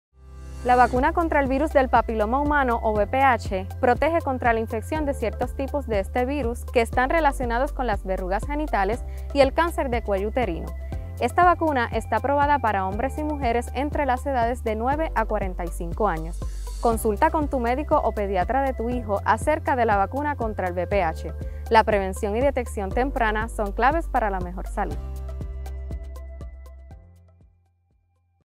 Vacuna contra el VPH 1
¿Cuál es la vacuna que protege contra ciertos tipos de virus relacionados con el cáncer de cuello uterino o las verrugas genitales? Presta atención a este video educativo.

Mes de la Concienciación de Cáncer de Cuello Uterino